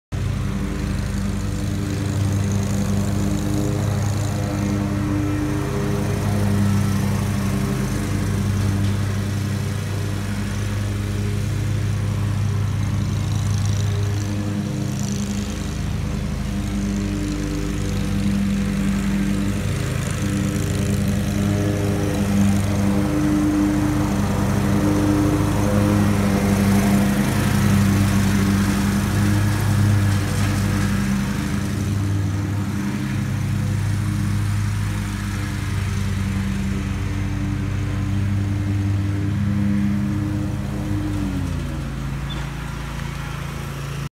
John Deere Z930M Tweel MOD
Mulching leaves back when we had a drought